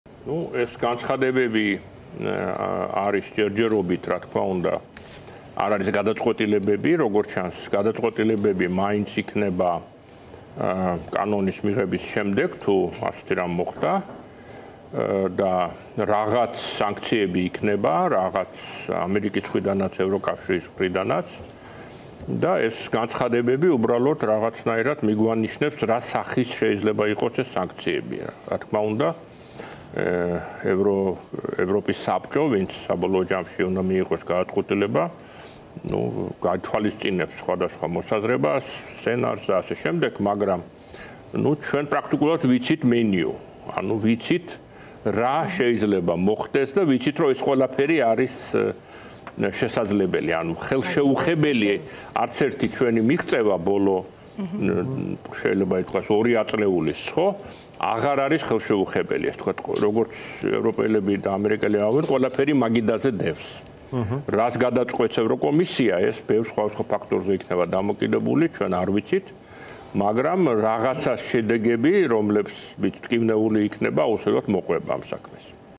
„თუ კანონს მიიღებენ, რაღაც სანქციები იქნება“ — გია ნოდია
▪️ „თუ [რუსულ] კანონს მიიღებენ, რაღაც სანქციები იქნება“ — გია ნოდია